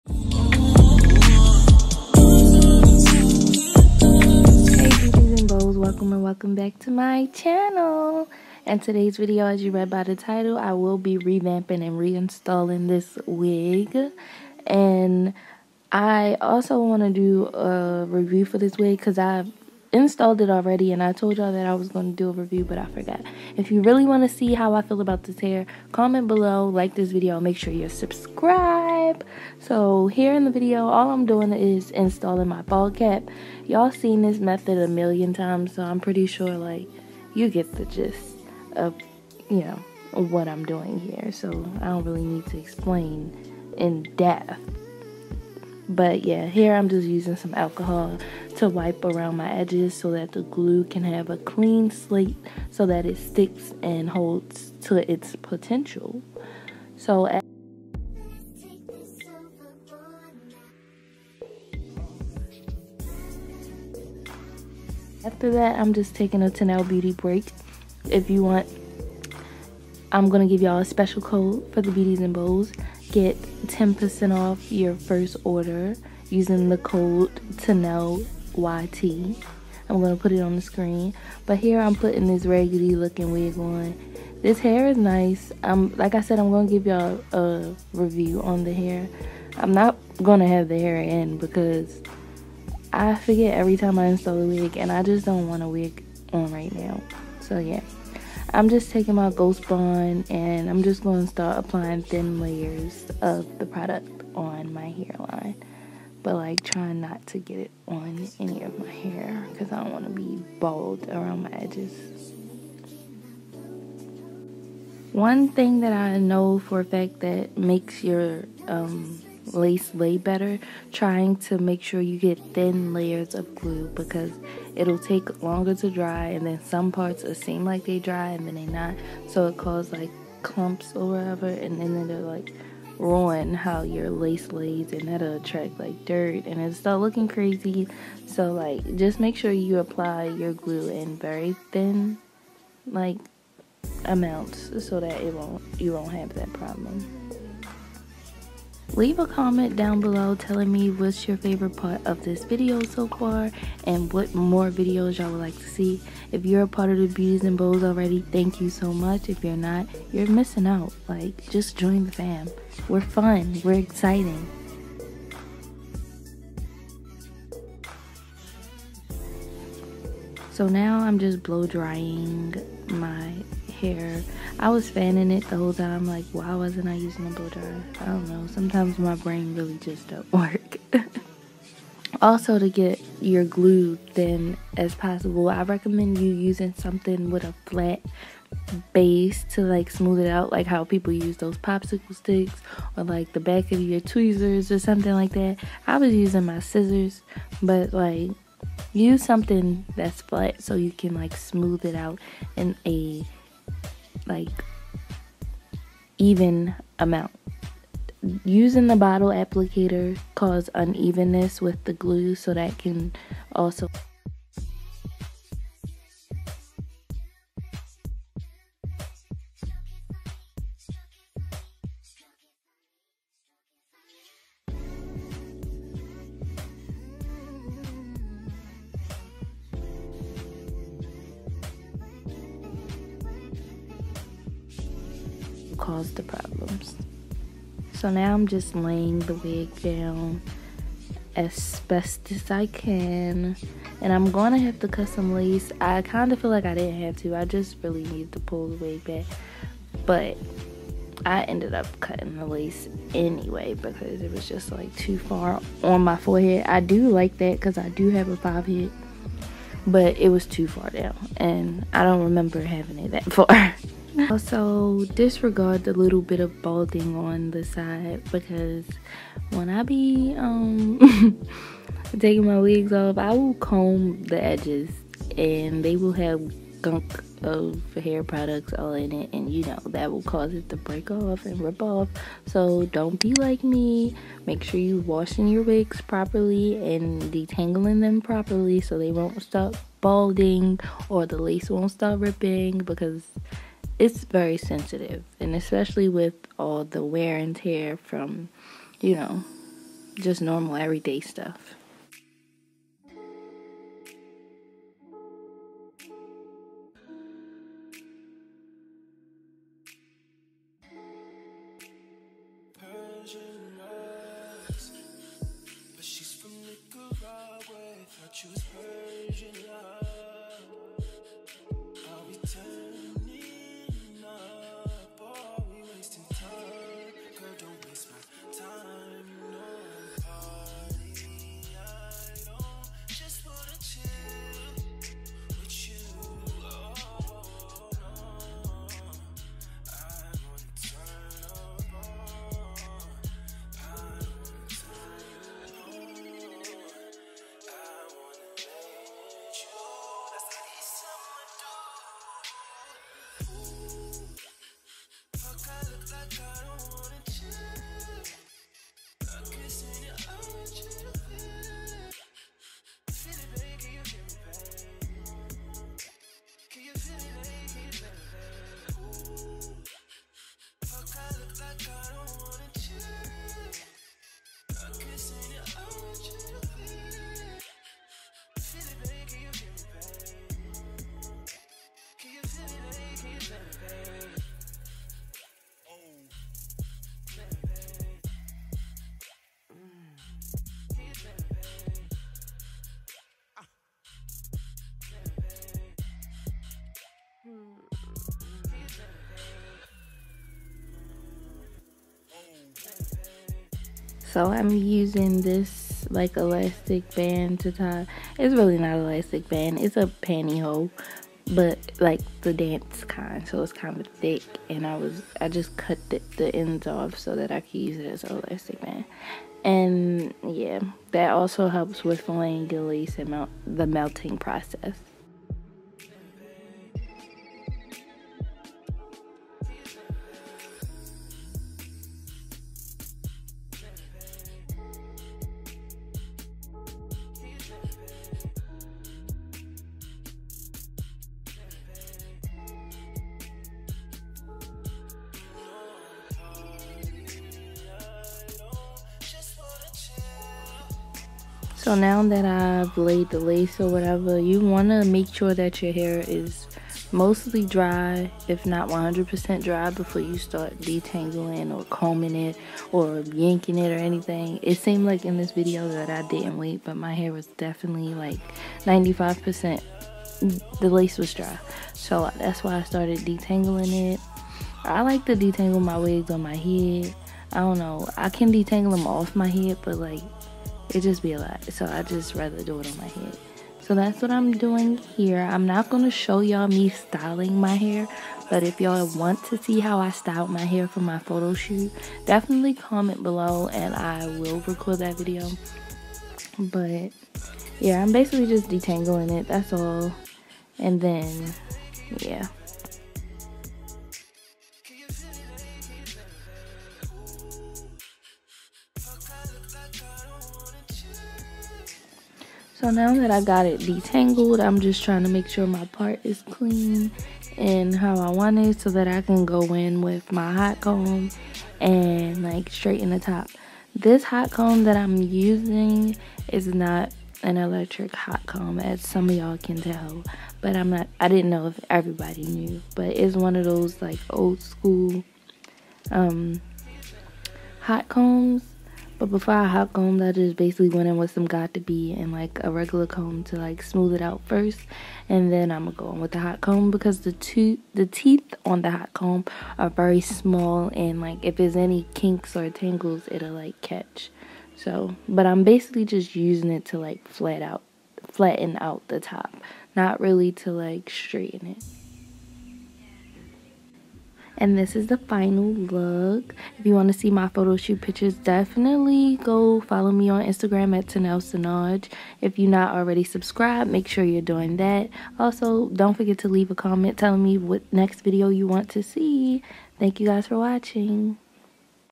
REVAMPING/REINSTALLING THIS WIG Ft (Pinketer Hair)
Hey beauties and beaus welcome or welcome back to my channel🥰 in this video you will see how I revamp this wig for my photo shoot, if you liked this video don’t for get to like, comment, and subscribe to my channel for more content like this💕